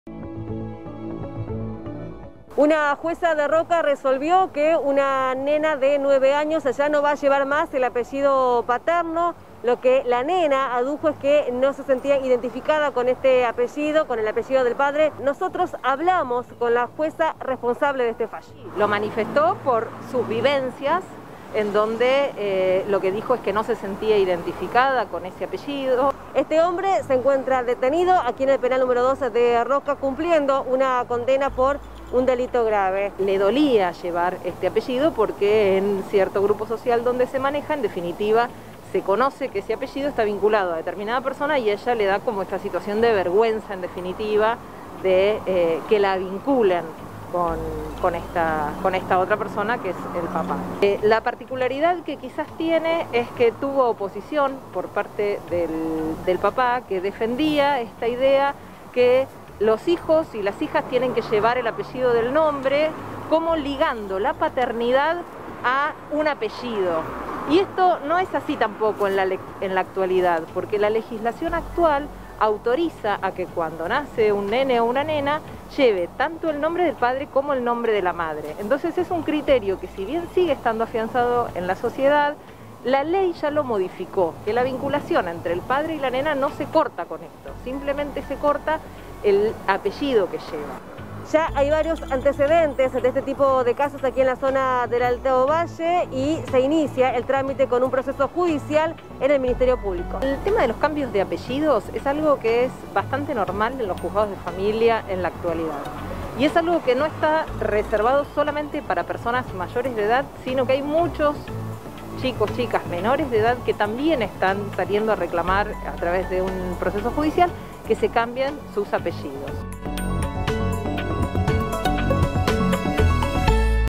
Noticias10 | Le otorgaron el cambio de apellido a una niña de 9 años
En General Roca se le reconoció el derecho a la identidad a una nena que no se sentía representada por el apellido paterno.